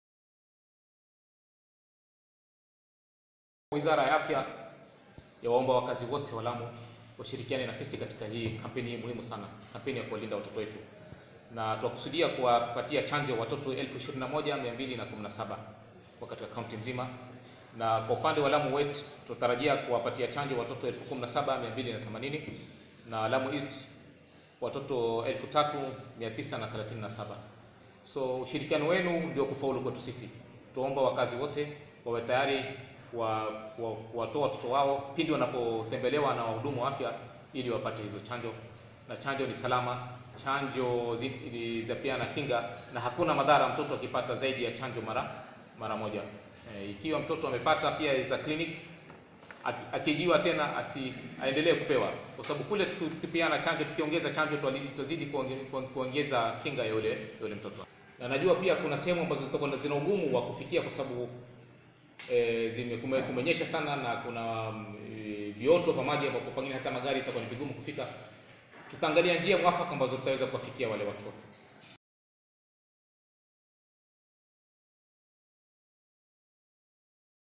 Polio campaign targets 21,217 children in Lamu
Lamu County Health Promotion officer Mohamed Muhsin says they are targeting at least 21,217 children of five years and below in the round 1 of polio vaccination campaign beginning Wednesday July 11 all the way to July 15.